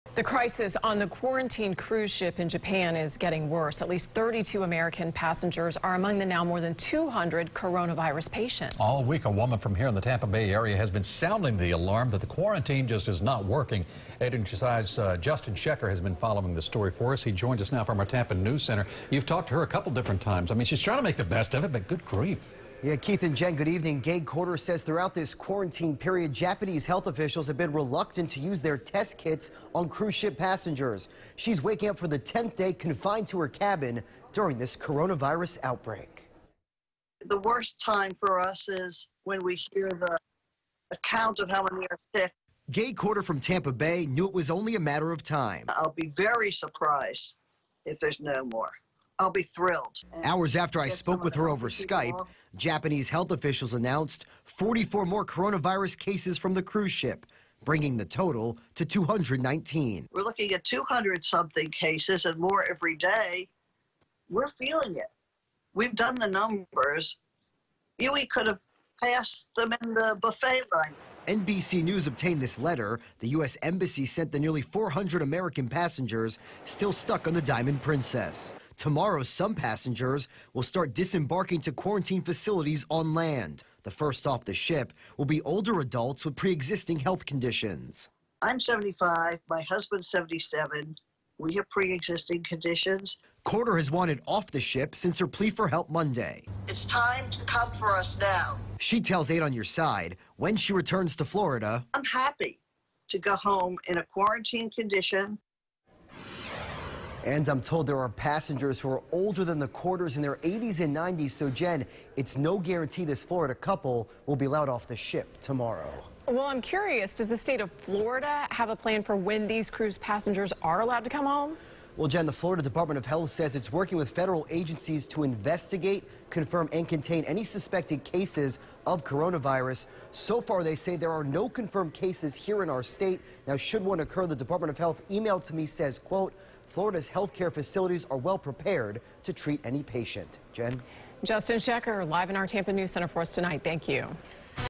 Cases of coronavirus top 200 as Florida couple awaits end of quarantine on cruise ship
Hours after 8 On Your Side spoke with Gay Courter over Skype, Japanese health officials announced 44 more coronavirus cases from the cruise ship being quarantined off Yokohama.